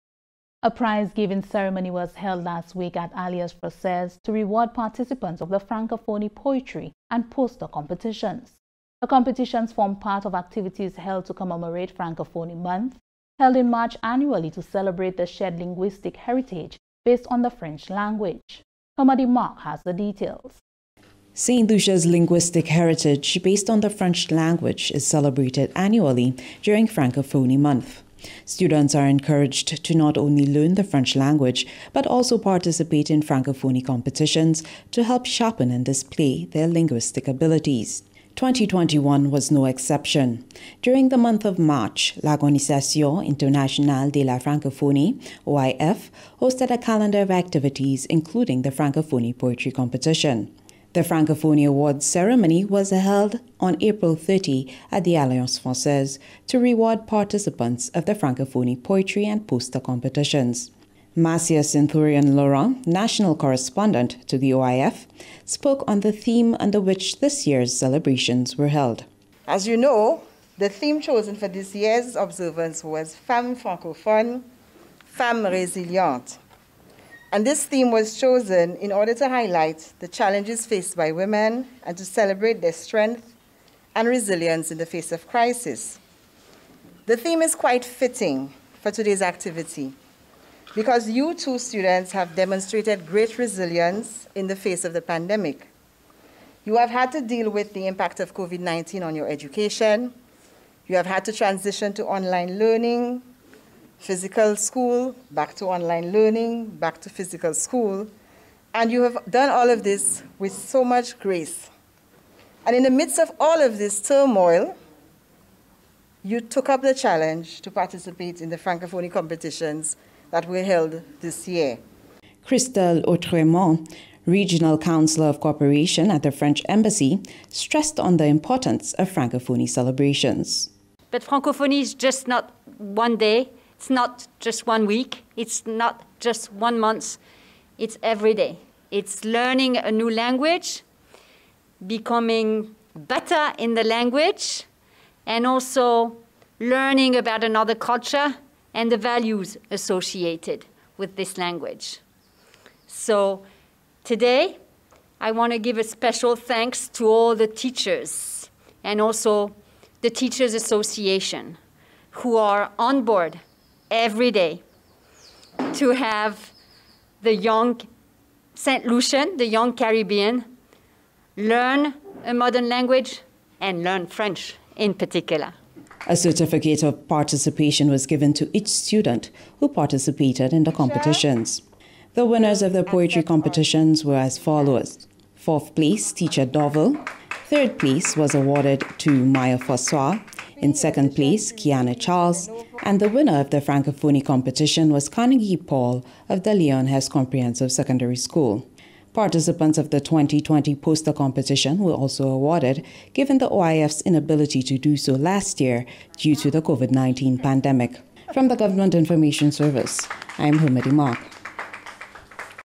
Francophonie Month prize giving ceremony for Poetry and Poster Competition
A prize giving ceremony was held last week at Alliance Francaise to reward participants of the Francophonie poetry and poster competition. The competitions formed part of activities held to commemorate Francophonie Month held annually to celebrate the shared linguistic heritage based on the French language.